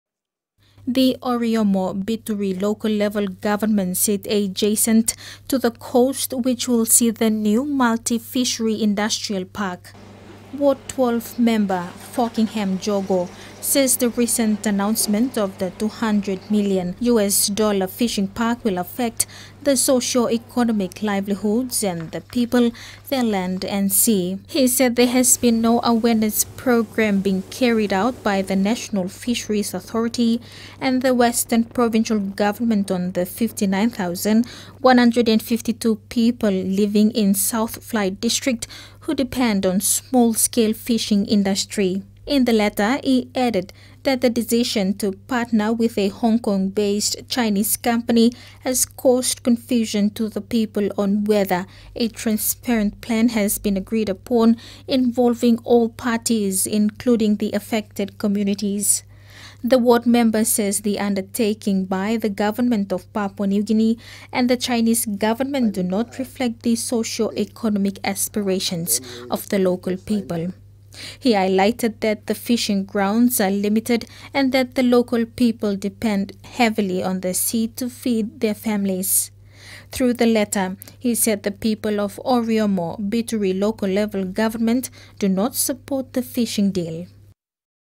Oriomo Bituri people
The local communities of Oriomo Bituri local level government of South Fly District have written to the Western Provincial Government outlining their grievances over a proposed 200 million US dollar industrial fishing park.

In the letter, the Ward 12 member explained that his people do not support the PNG-Chinese fishing deal.